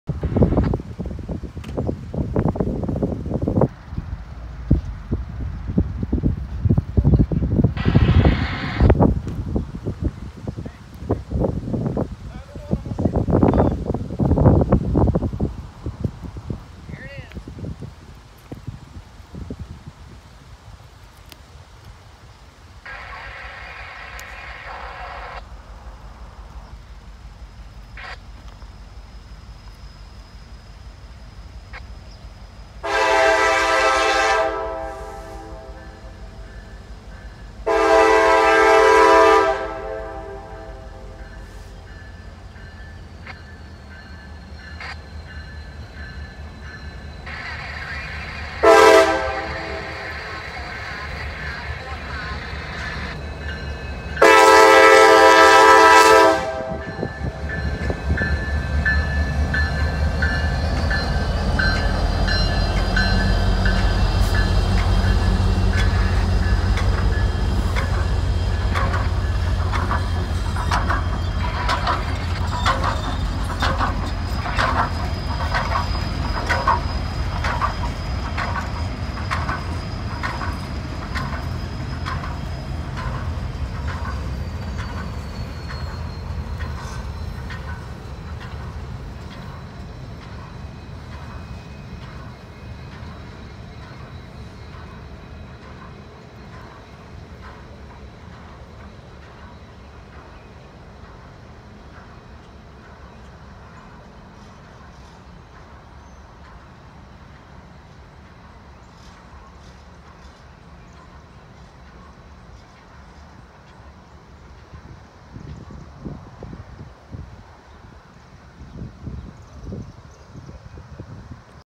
N&W 2156 Leaves Virginia for St. Louis
Here we see NS 911 (with EMD 7030 as power) towing the N&W 2156 west at Elliston, VA on Wednesday, June 10 at 5:40 p.m. as it heads back to St. Louis, Missouri.  Video shot in 4k.